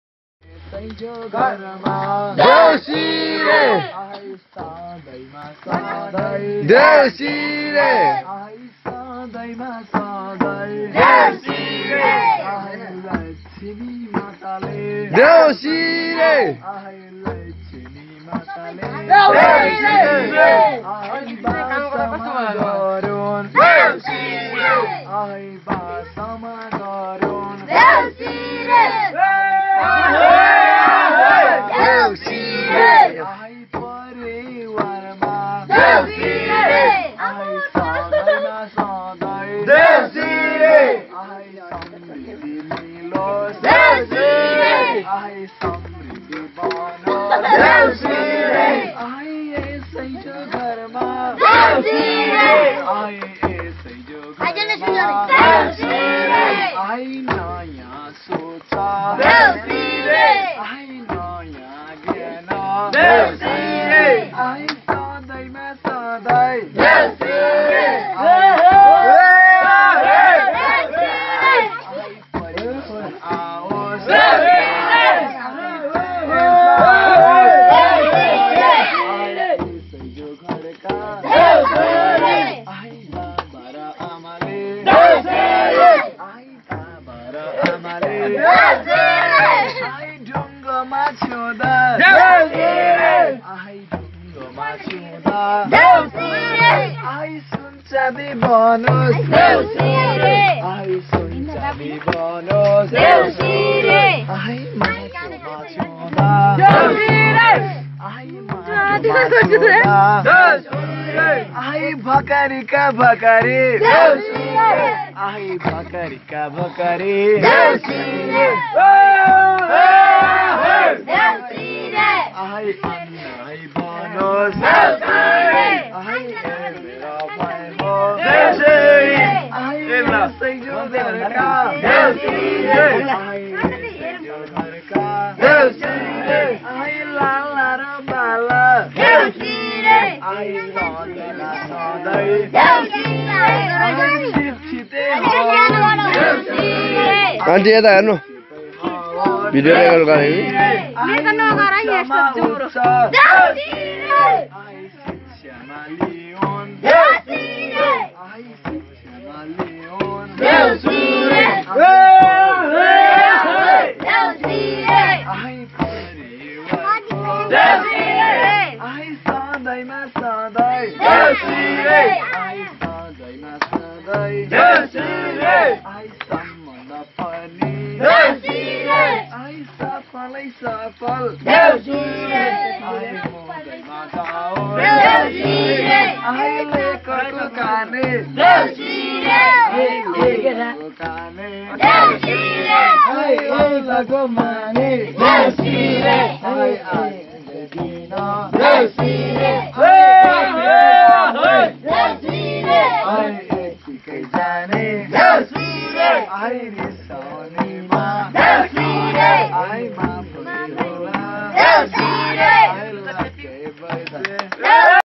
Bhatyaune program 2076 at tirpa
Anoj Rijal as well as all player are playing deusi at man dai house Camera :Sudip Parajuli -2076
Madal: Ramesh kc